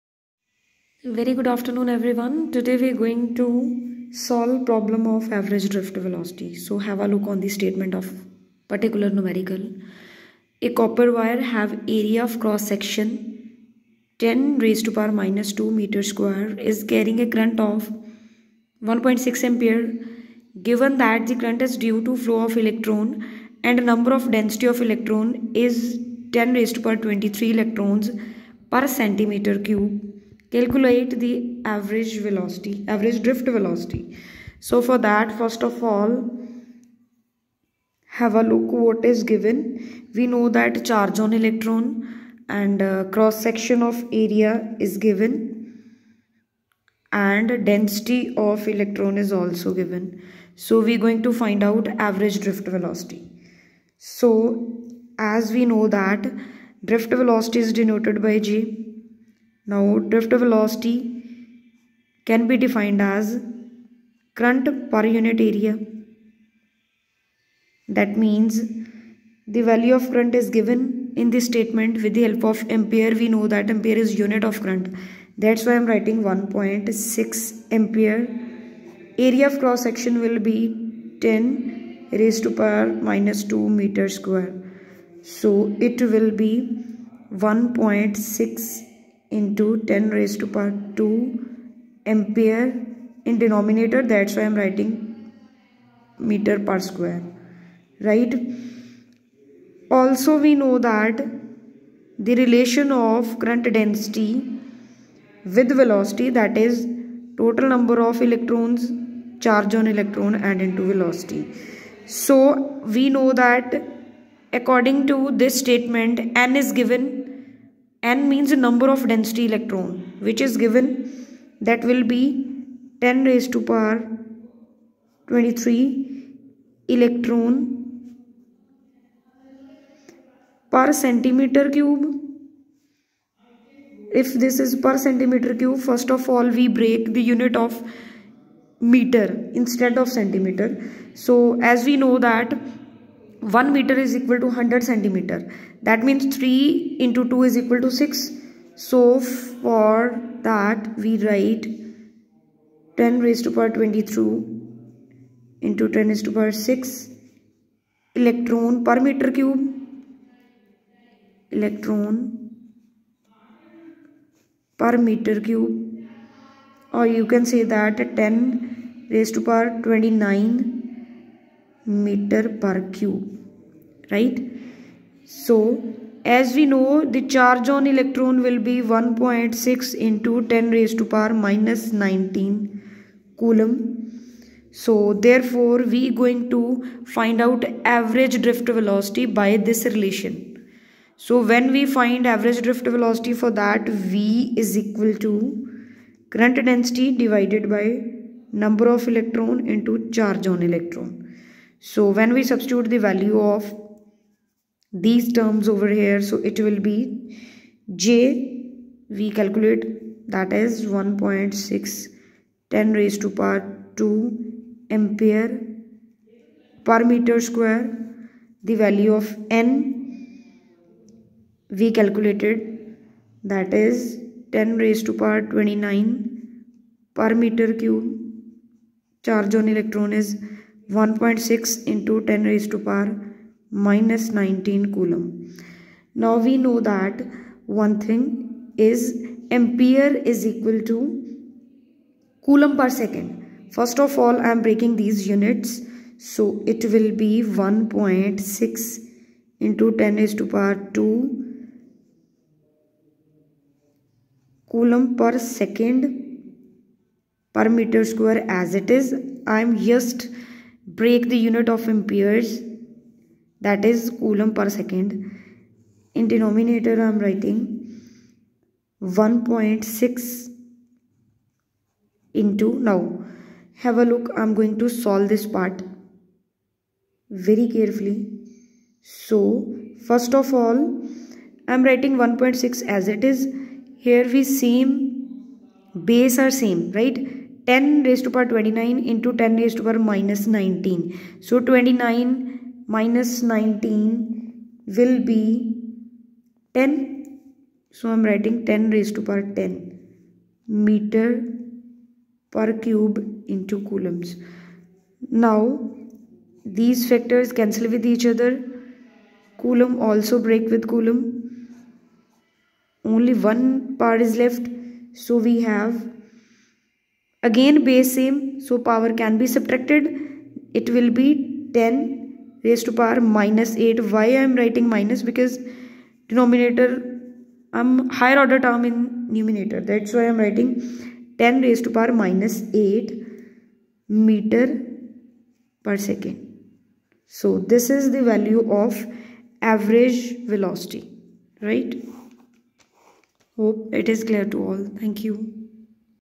how we calculate drift velocity.
how we calculate drift velocity. Numerical problem of Average drift velocity.